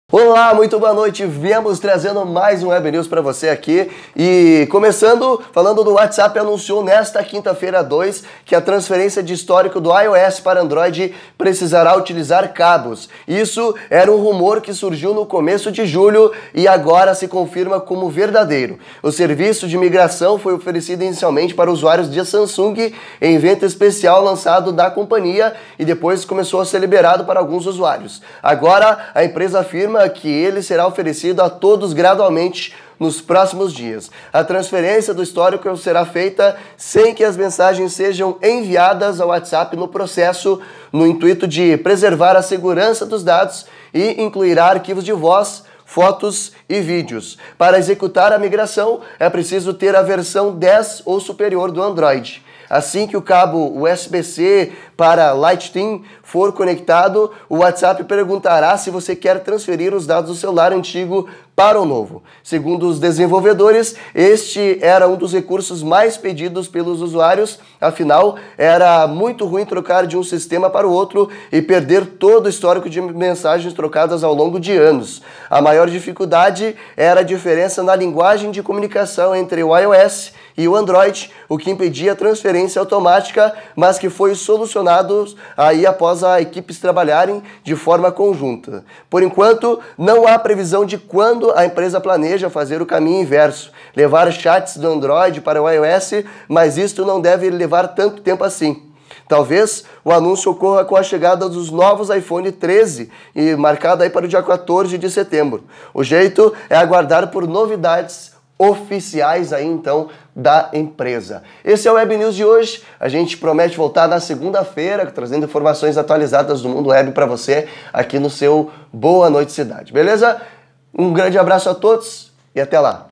WhatsApp confirma que transferência de histórico de conversas exigirá cabo
O WhatsApp anunciou nesta quinta-feira (2) que a transferência do histórico do iOS para o Android precisará utilizar cabos — isso era um rumor que surgiu no começo de julho e agora se confirma como verdadeiro. O serviço de migração foi oferecido inicialmente para usuários da Samsung, em evento especial de lançamento da companhia, e depois começou a ser liberado para alguns usuários. Agora, a empresa afirma que ele será oferecido a todos gradualmente nos próximos dias.